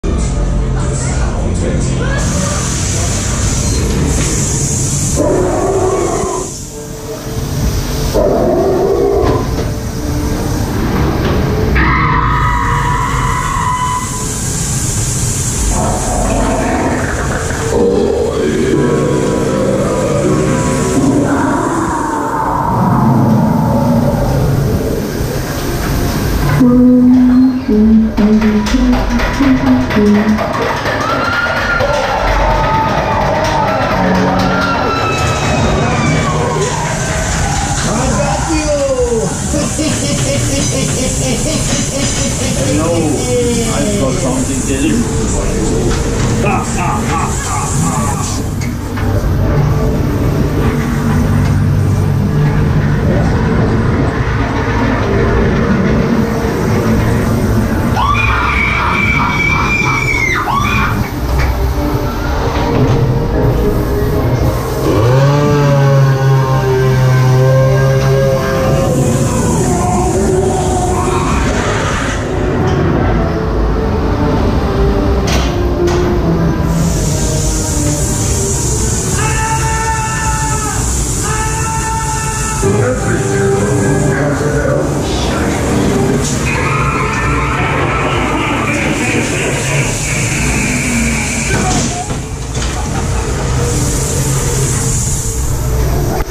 Haunted Mansion - Hinzen (ONRIDE) Herbst-SEND Münster 2021
Hersteller: Gosetto
Typ: Suspended Ghost Train

Fahrpreis: 5€